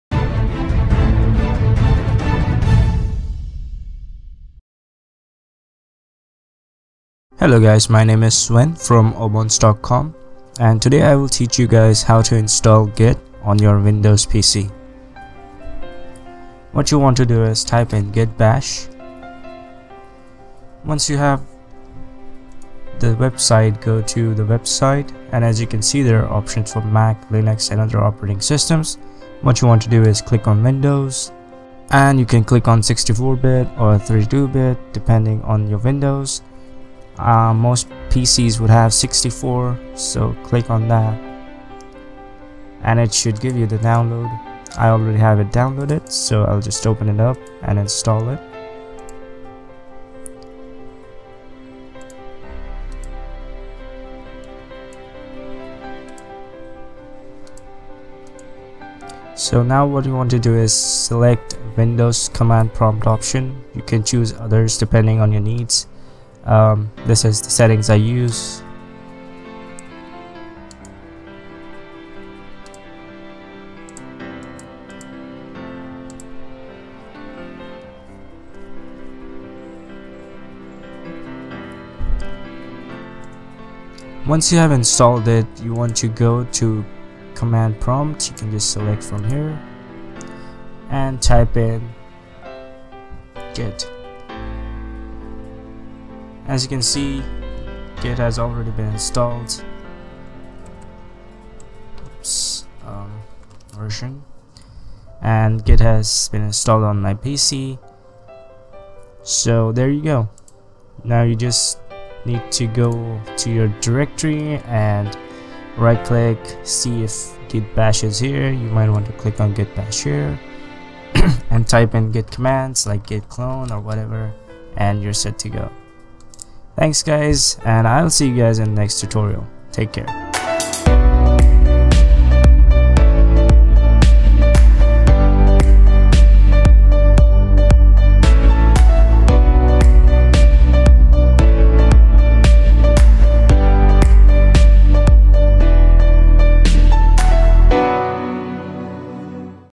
How to install Git on Windows 7,8,10 Very Easily
Hello guys, I'm Swen from Obonce.com, in this tutorial I will show you how you can install Git on Windows 7,8,10 very easily.